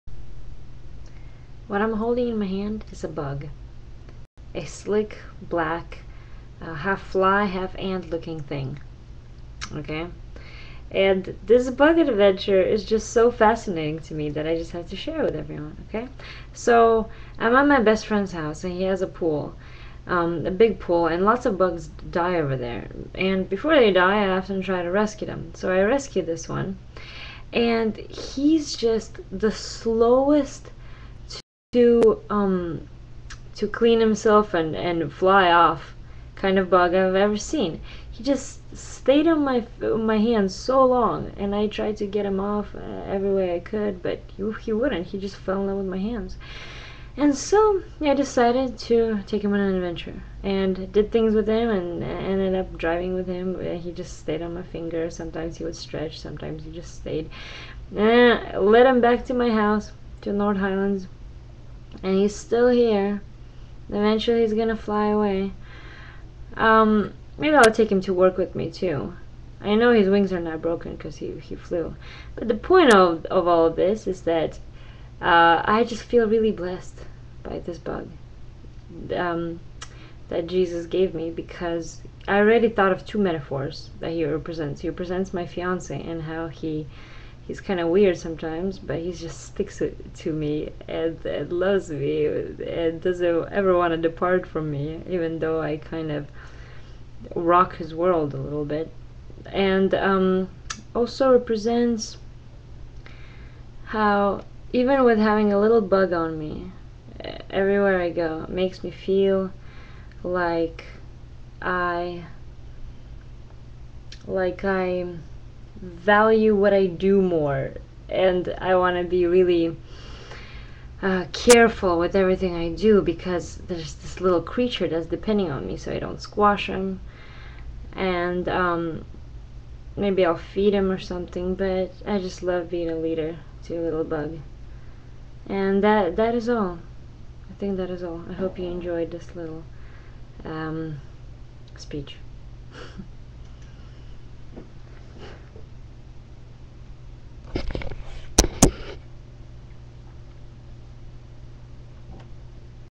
""A Bug Adventure""
Maybe its a special bug that only lives on one branch all of it's life, or maybe its a female that plans to lay eggs on my hands.